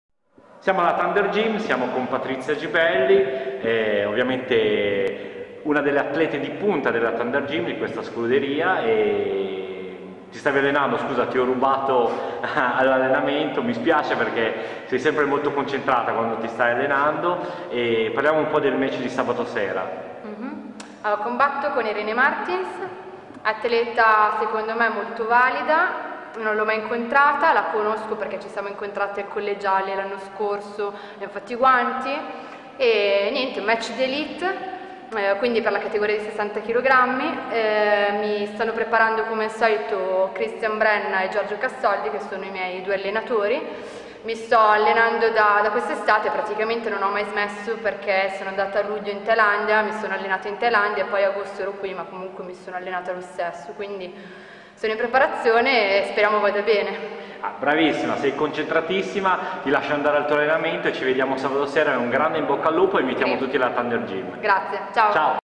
Patrizia "Valkyrja" Gibelli - Inside The Thunder 4 (Milano Ottobre 2013)
Patrizia "Valkyrja" Gibelli intervistata durante una sessione di allenamento in preparazione dell'incontro con Irene Martens che si terrà a Inside The Thunder 4, l'evento dedicato agli Sport da Ring a contatto pieno dilettanti e PRO.

La location è ancora la Thunder Gym Milano, all'insegna dello Sport, della musica e del divertimento. L'evento avrà inizio alle h 15.00 con match dilettanti di tutte le discipline da combattimento, al termine dei combattimenti pomeridiani ci sarà un aperitivo con buffet per tutti i presenti con un fantastico allestimento all'interno della struttura, per poi riprendere intorno alle h 20.30 con il Galà serale tra combattimenti, musica, spettacoli e drink. Nella serata si svolgeranno le selezioni delle ring girls per la stagione WSB 2013/2014 dei Dolce&Gabbana Italia Thunder.